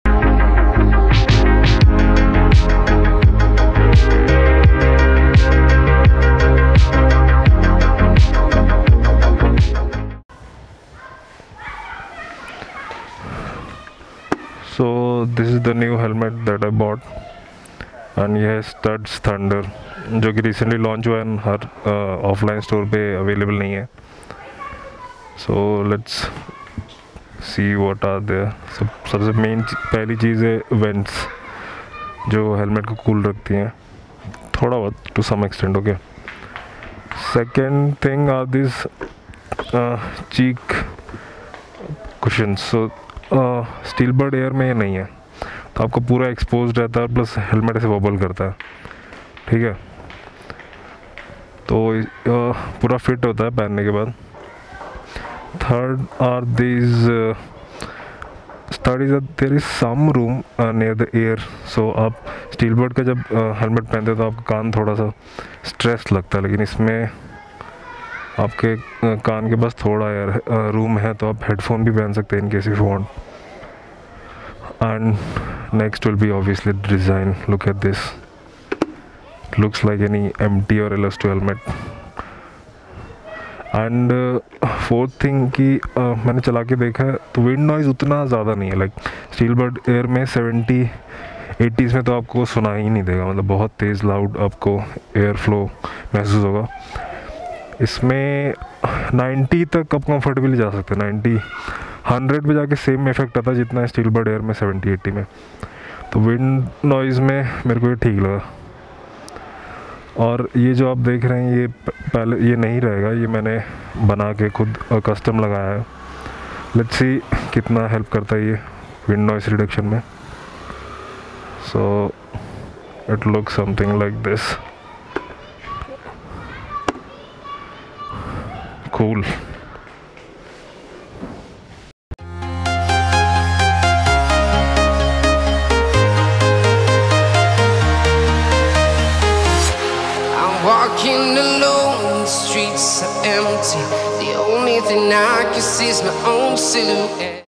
Studds THUNDER | Better than SteelBird Air
Short Helmet Review of Studds Thunder. Looks good. Fits good. Priced good.